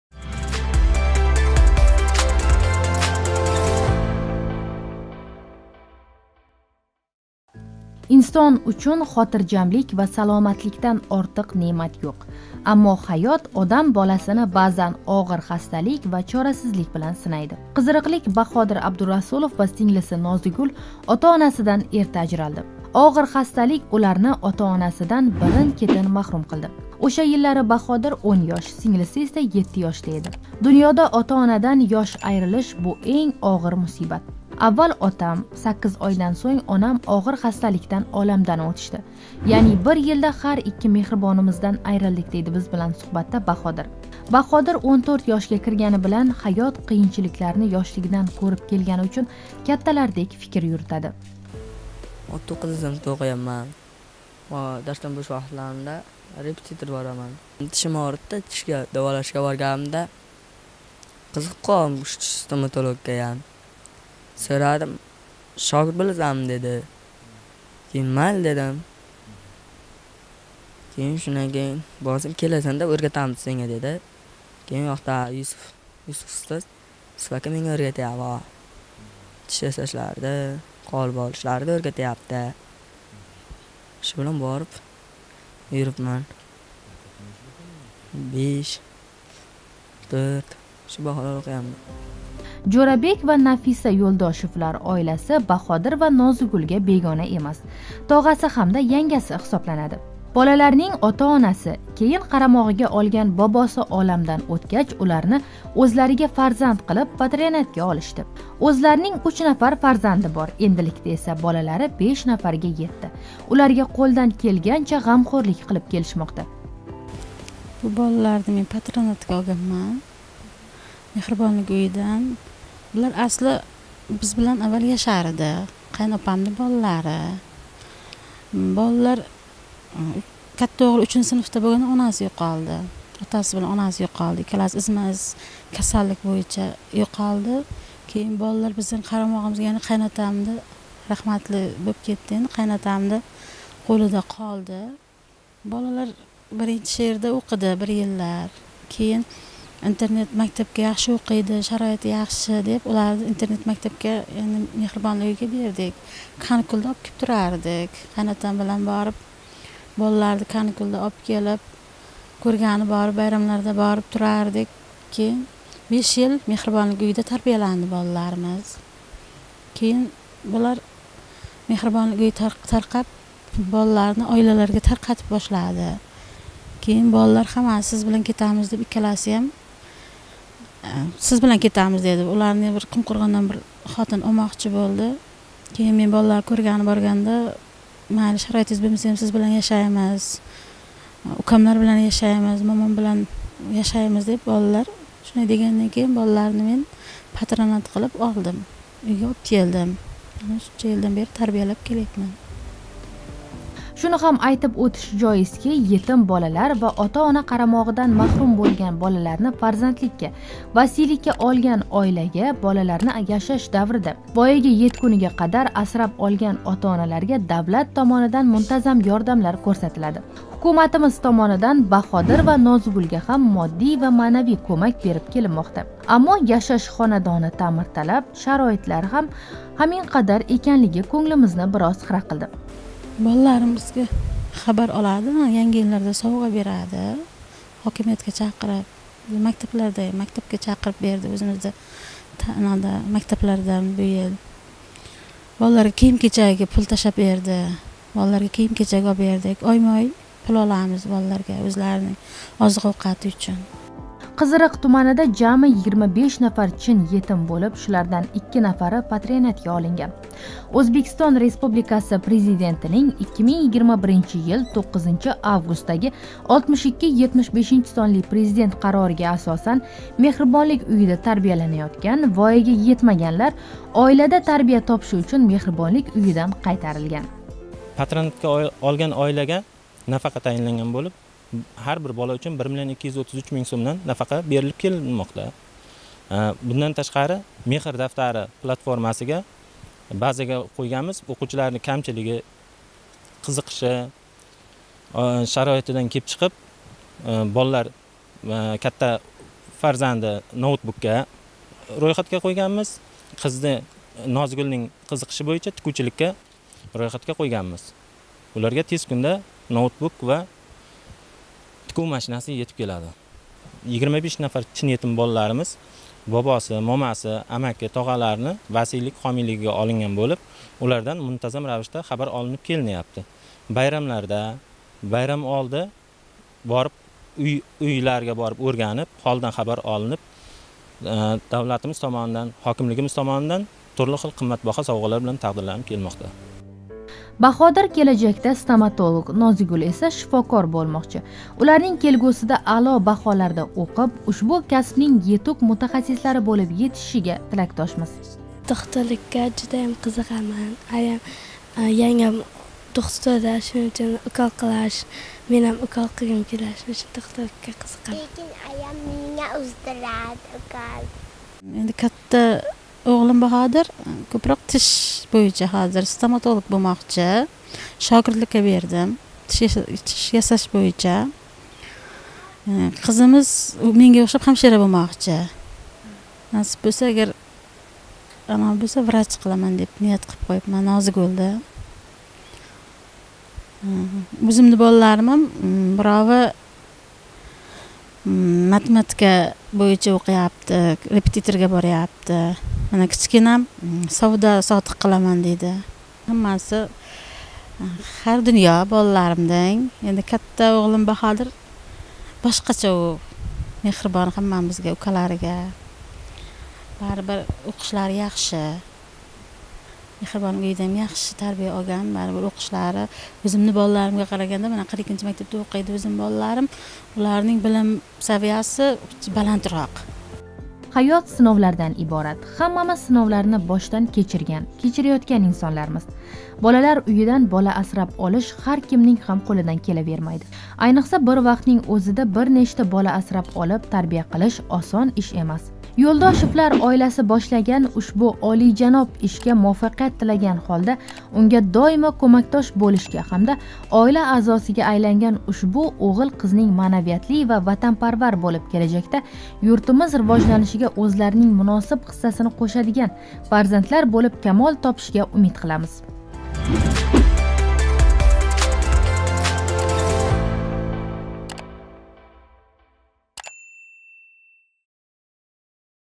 Нафиса ва Жўрабекни қизириқлик Йўлдошевлар фарзандликка олишди
Қизириқлик Баҳодир Абдурасулов ва синглиси Нозигул ота-онасидан эрта ажралди. Оғир хасталик уларни ота-онасидан бирин –кетин маҳрум қилди. Ўша йиллари Баҳодир 10 ёш, синглиси эса 7 ёшда эди.

Жўрабек ва Нафиса Йўлдошевлар оиласи Баҳодир ва Нозигулга бегона эмас, тоғаси ҳамда янгаси ҳисобланади. Болаларнинг ота-онаси, кейин қарамоғига олган бобоси оламдан ўтгач, уларни ўзларига фарзанд қилиб (патрионатга) олишди.